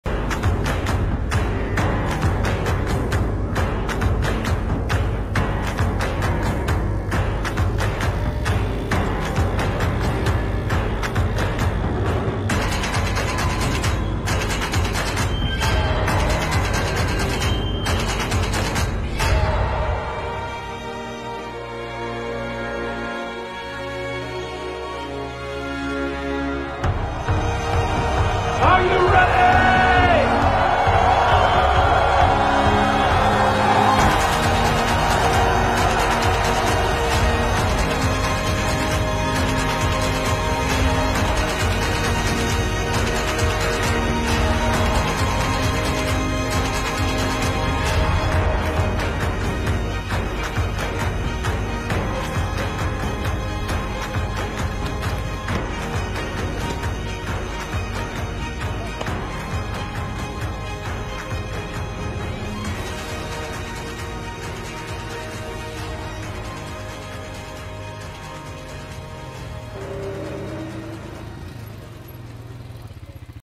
Riding ATVs across the Sahara desert of Morocco
Riding ATVs across the Sahara desert of Morocco, our dynamic and knowledgeable team is always encouraging our guests to be passionate and to indulge the adrenaline.